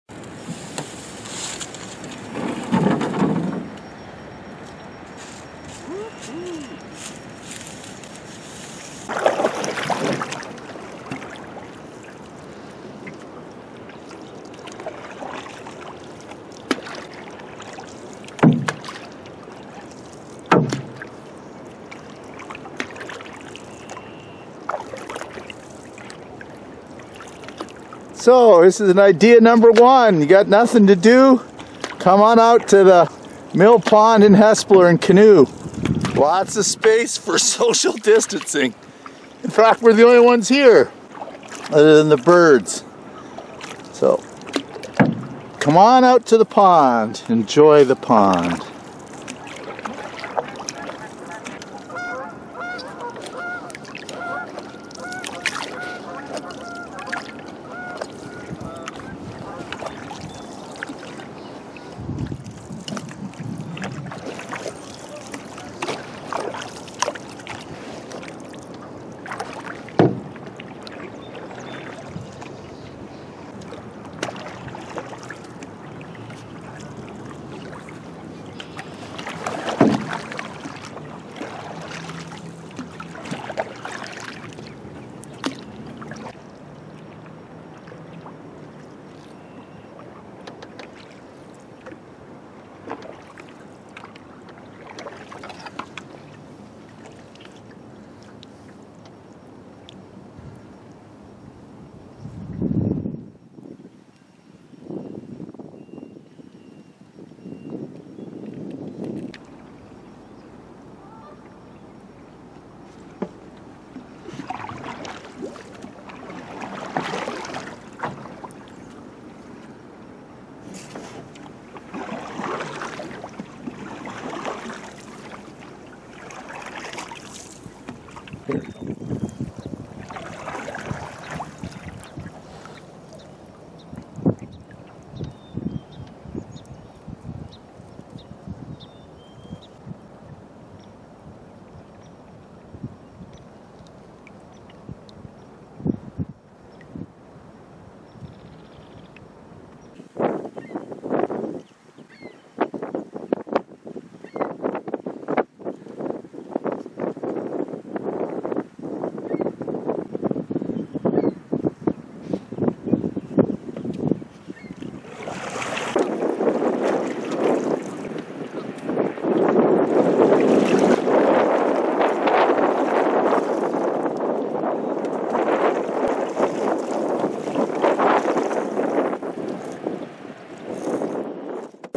Canoeing on the Hespeler Mill Pond, Speed River, March 25, 2020
A great healthy activity for the whole family. We did not have much of a winter so the pond is already shallow. Stay along the outside of the pond and enjoy the beauty. Parking at lot at the end of Spring Street. The dock may or may not be in. Does not matter you can enter your canoe in at Little Riverside Park, which starts at the end of Spring Street. Simply beautiful.